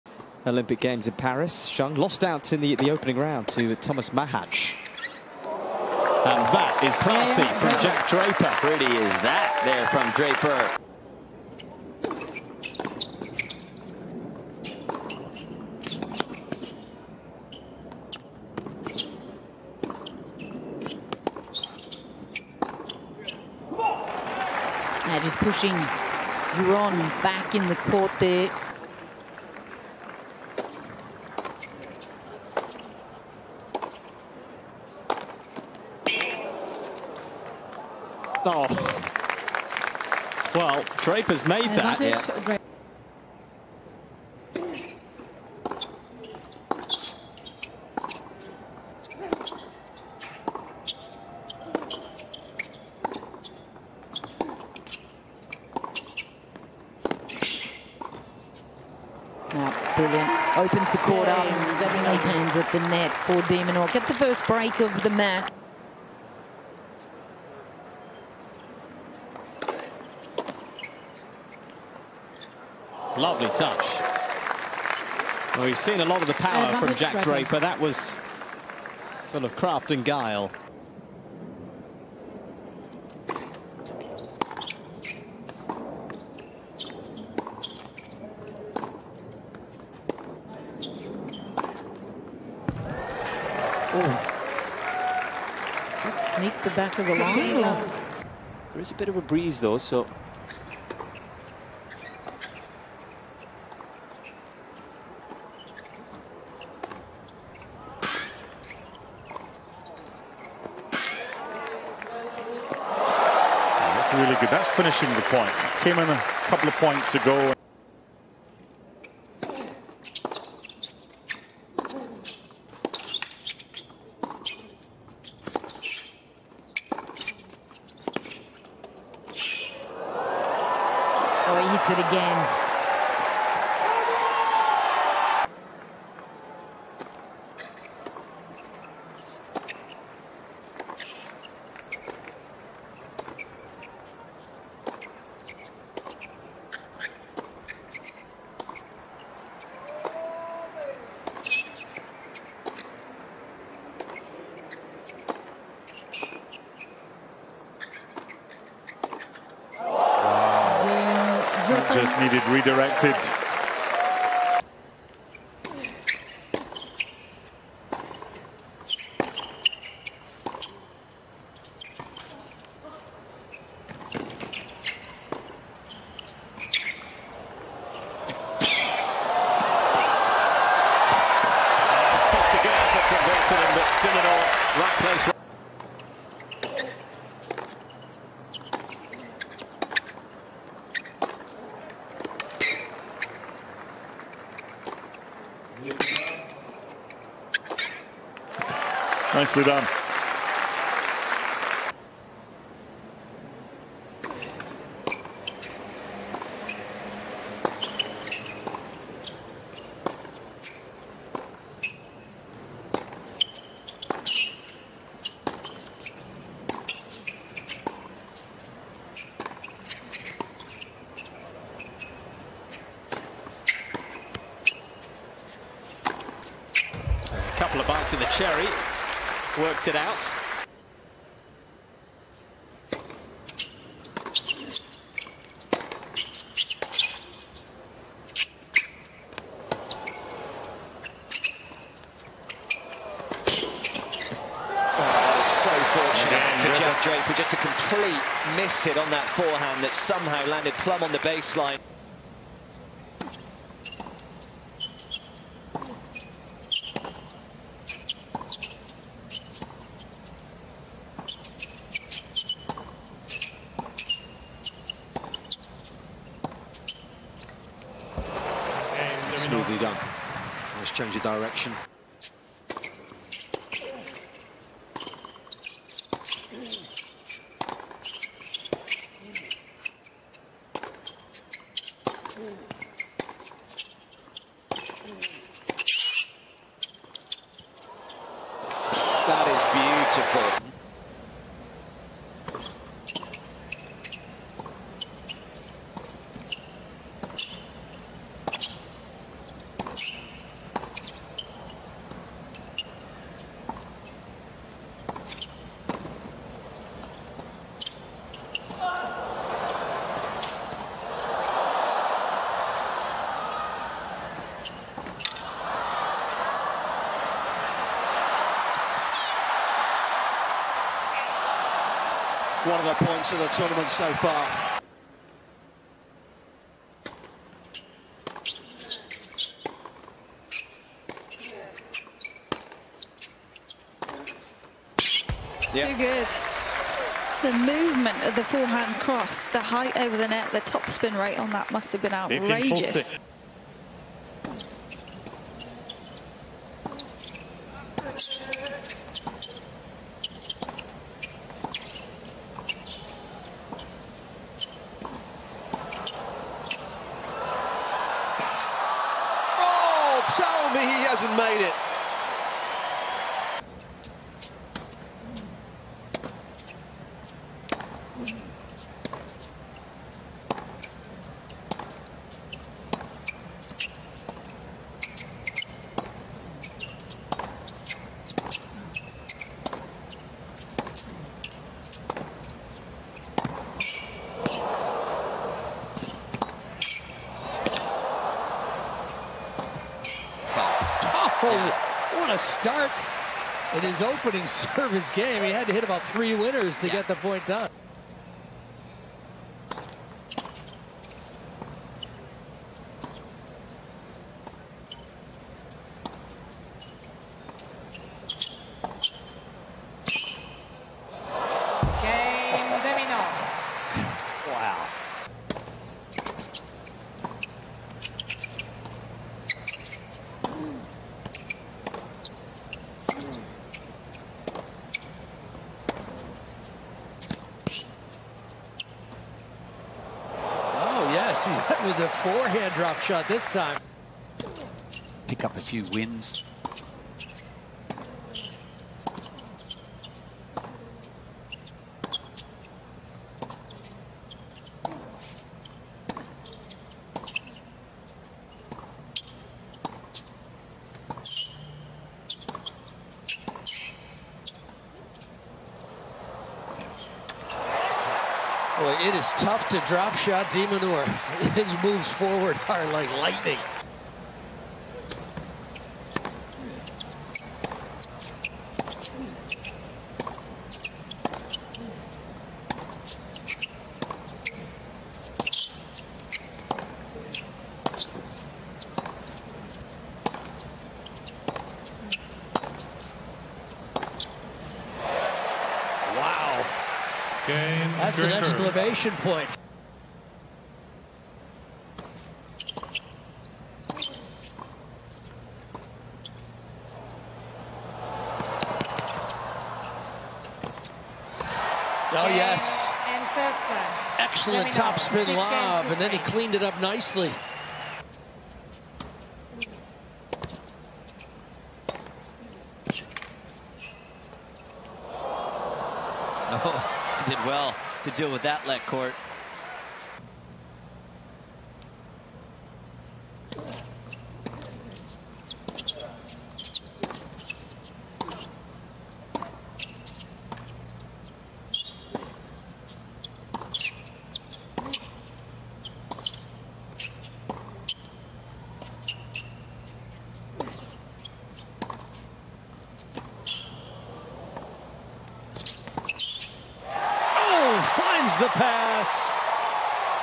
Jack Draper vs. Alex de Minaur | Road to the Quarterfinals | 2024 US Open
Jack Draper will face Alex de Minaur in the quarterfinals of the 2024 US Open.